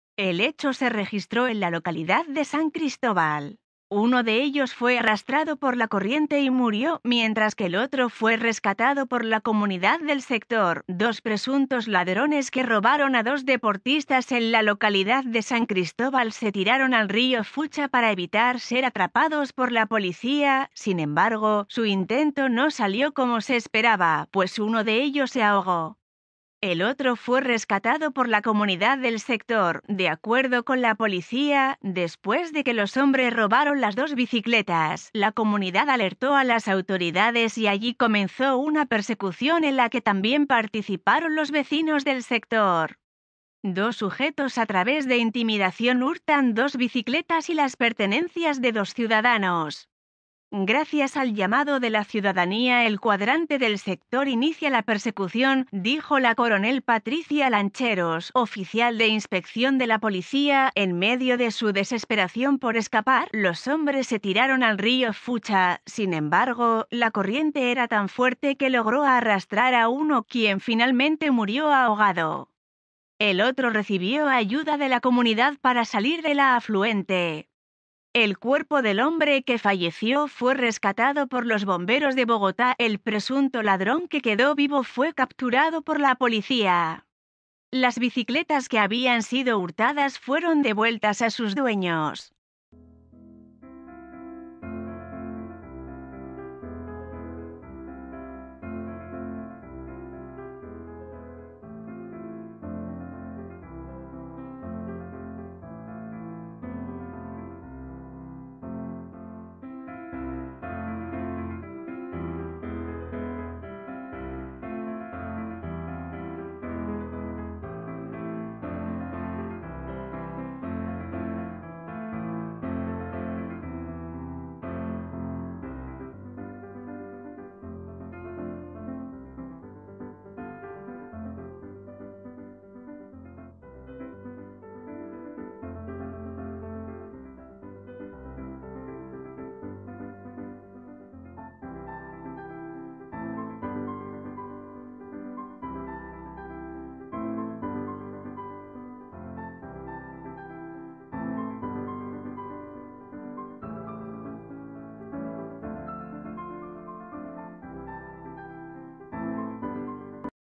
Presuntos ladrones se tiraron a un río para escapar de la Policía, en Bogotá
El hecho se registró en la localidad de San Cristóbal. Uno de ellos fue arrastrado por la corriente y murió, mientras que el otro fue rescatado por la comunidad del sector.Dos presuntos ladrones que robaron a dos deportistas en la localidad de San Cristóbal se tiraron al río Fucha para evitar [...]